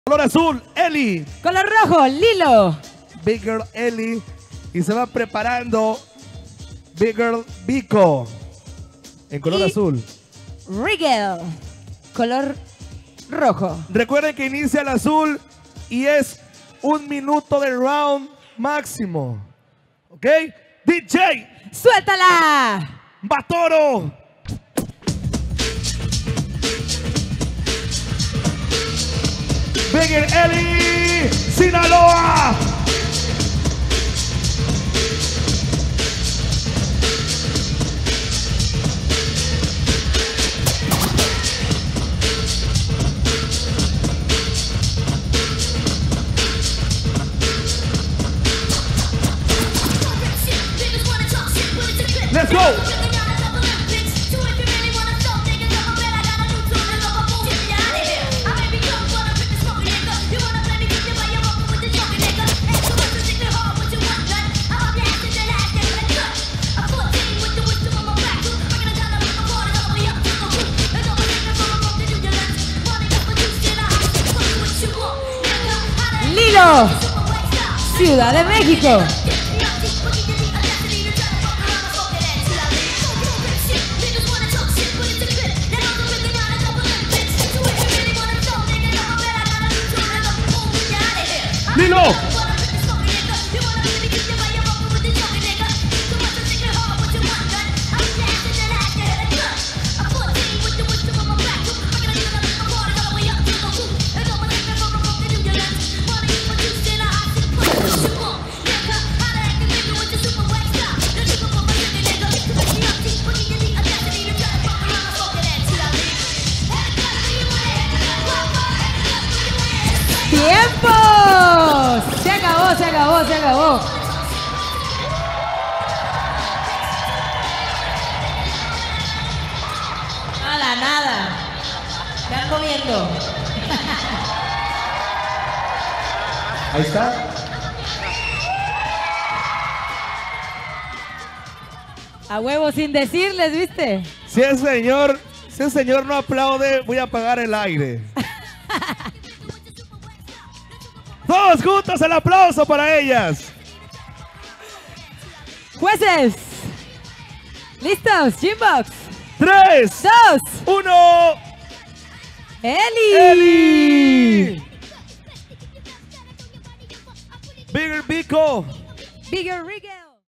TOP 32 B-Girls Juvenil | Ely vs Lilo | Nacionales CONADE | TABASCO 2023
Esta fue la batalla número 6 del TOP 32 de la categoría B-Girl Juvenil de los Juegos Nacionales CONADE en Villahermosa, Tabasco 2023.

Tenemos del lado azul a B-Girl Ely representando a Sinaloa y del lado rojo a B-Girl Lilo representando a Ciudad de Mexico.

Ganadora: B-Girl Ely de Sinaloa.

  Nos estamos preparando para llevar el Breaking a las Olimpiadas Paris 2024!.

Créditos
Mosk Mx: